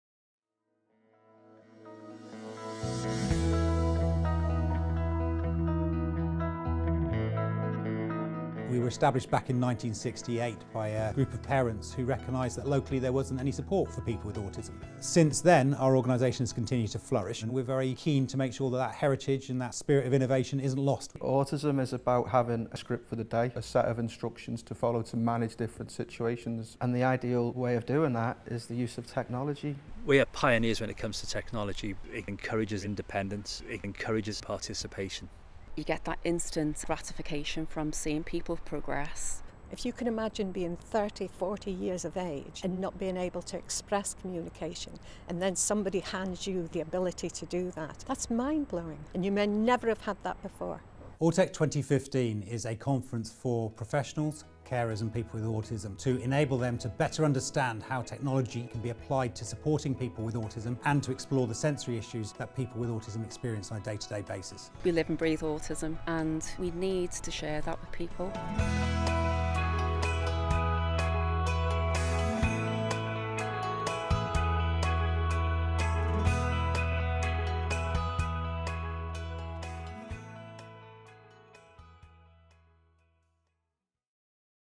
Autech 2015 - Assistive Technologies and Sensory Issues Conference
Autech//2015 - Assistive Technologies and Sensory Issues Conference - October 1st, Old Trafford Manchester.

Learn about assistive technologies available now. Gain an insight into what’s coming in the future. Be the first in the UK to hear US scientist Matthew Goodwin speak about biometric technology.

A new conference from multi-award winning charity Wirral Autistic Society, designed to inform and inspire anyone interested in assistive technology in the field of autism. The conference will also explore sensory issues.